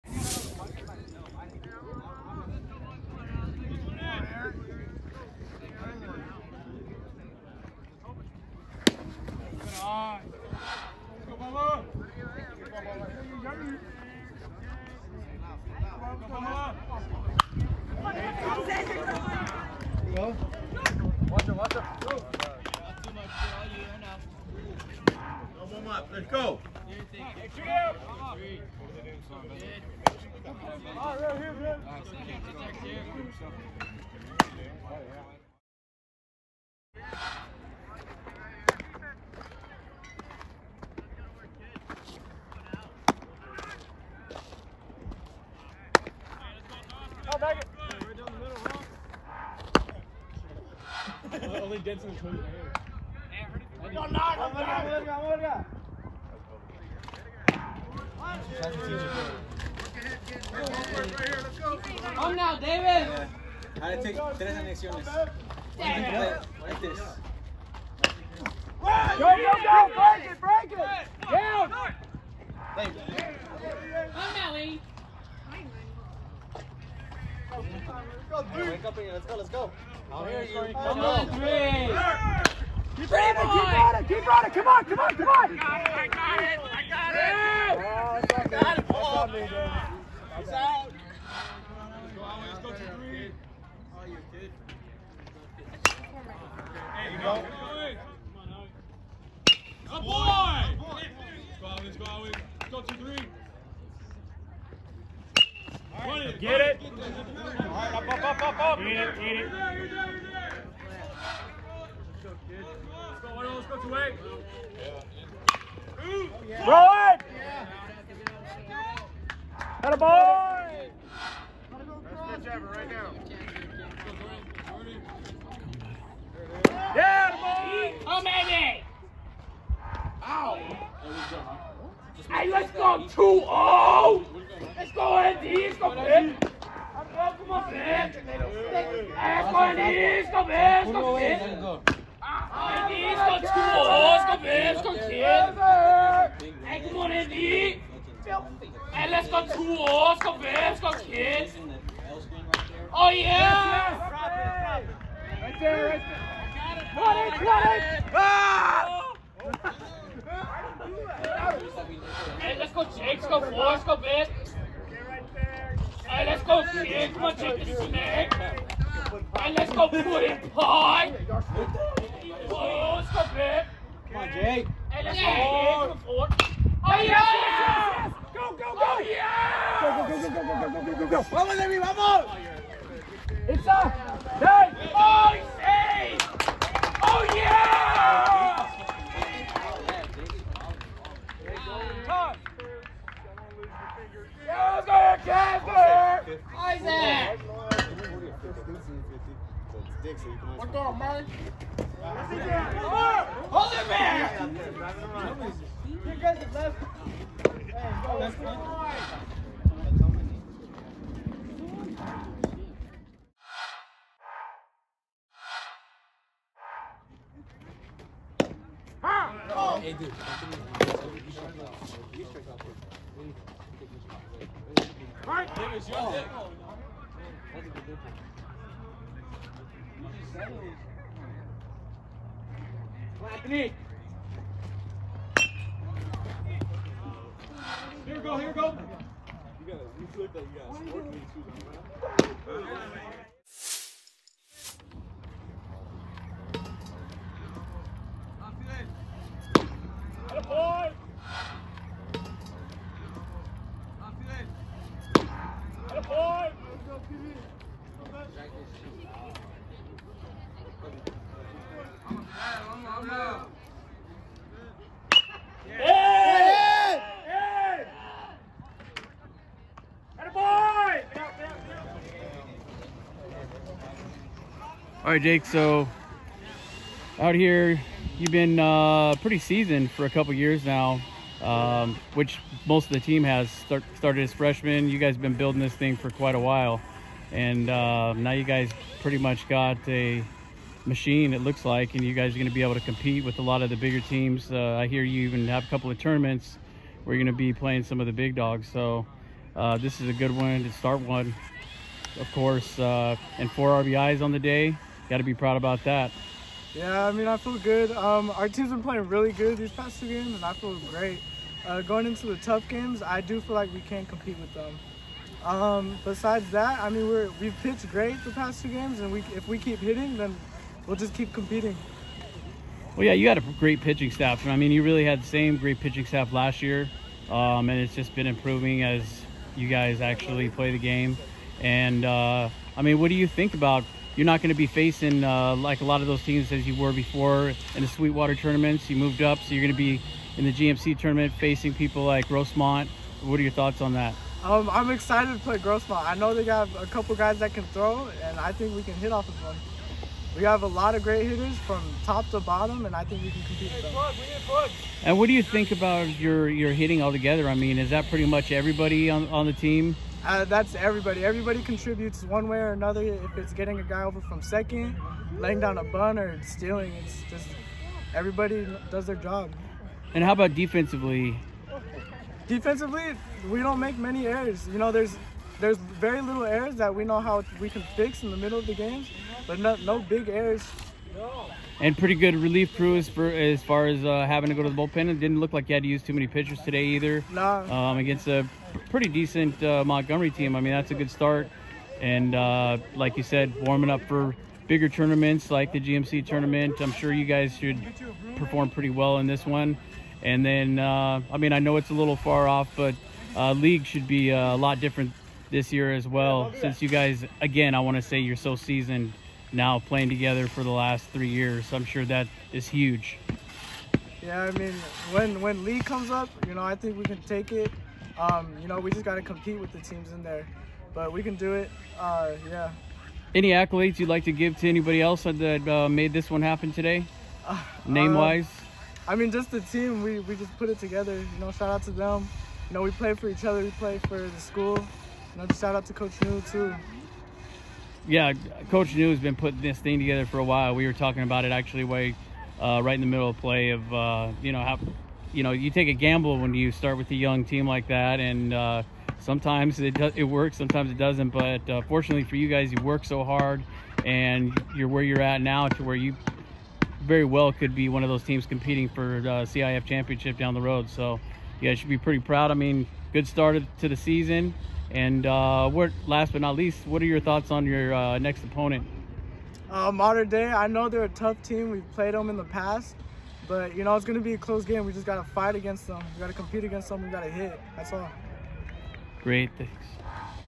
MONTGOMERY VS MONTE VISTA VARSITY BASEBALL - Feb. 26, 2020
Video by Dave Dickens